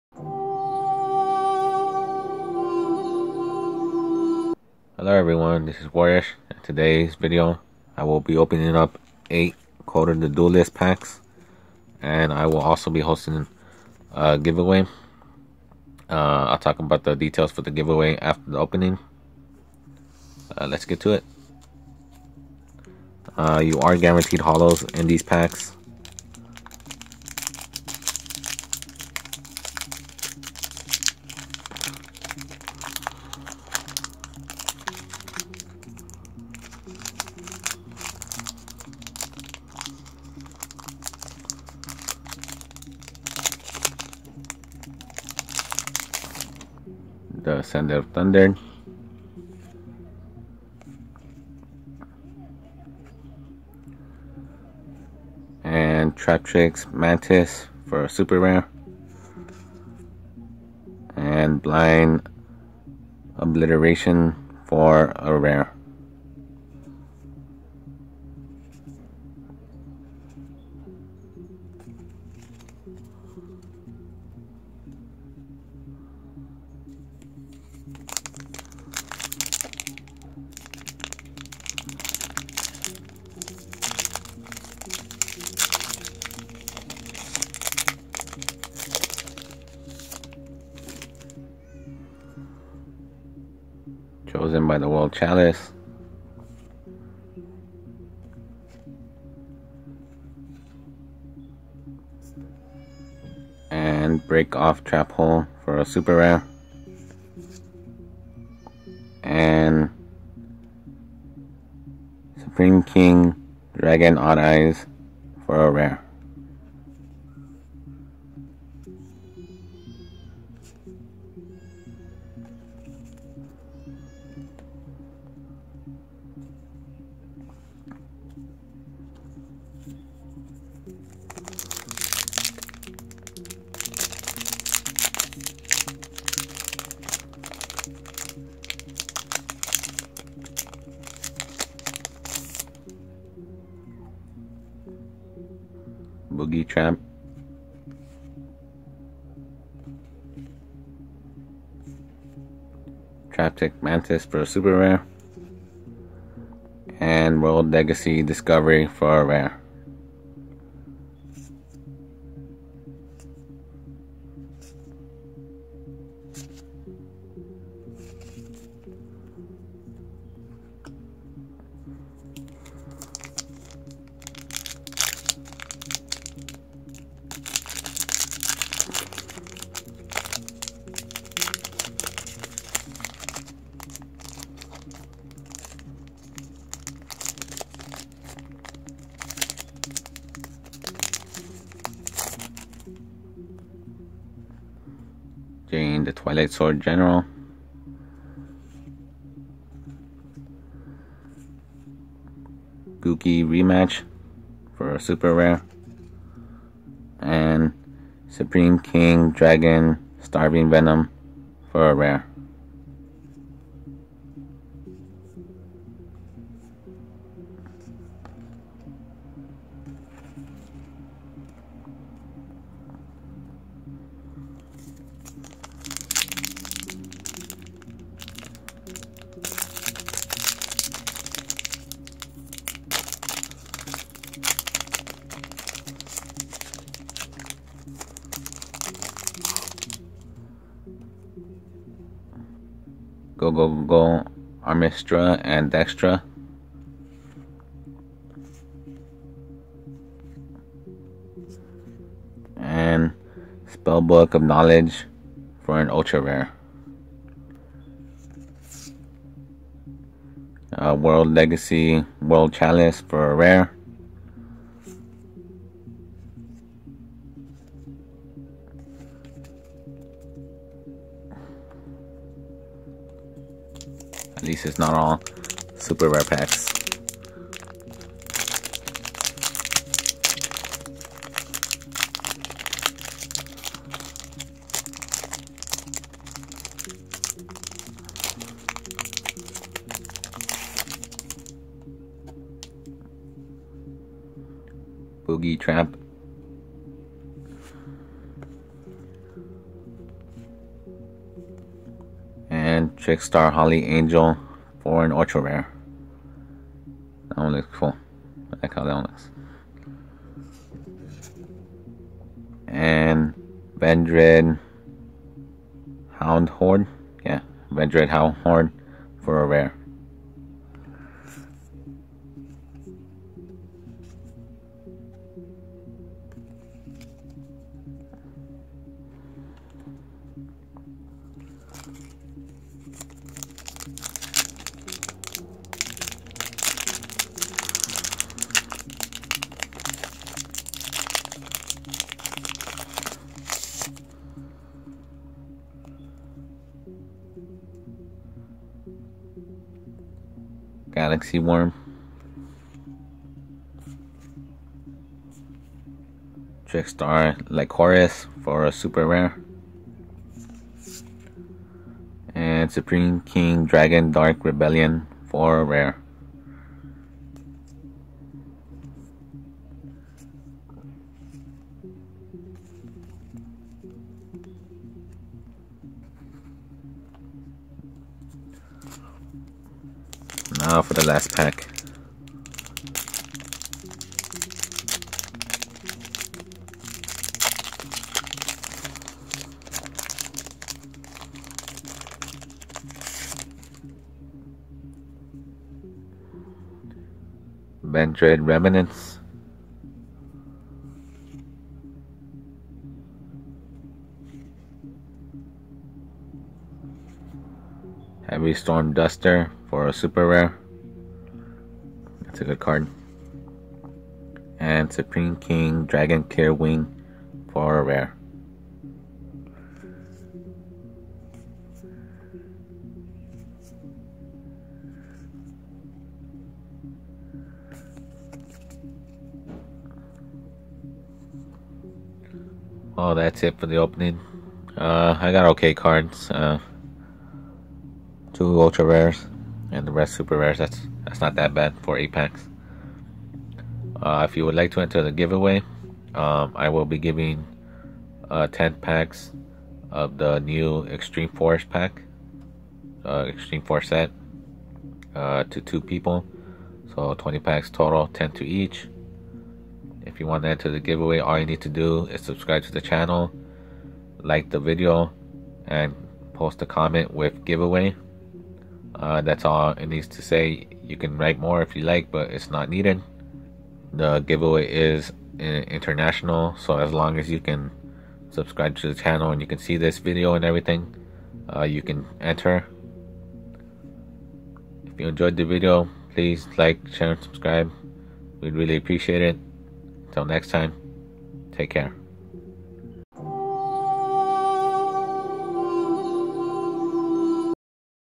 Yu-Gi-Oh! Code of the Duelist pack x8 opening & Giveaway.
My Yu-Gi-Oh! Code of the Duelist pack opening. What other openings would you want to see?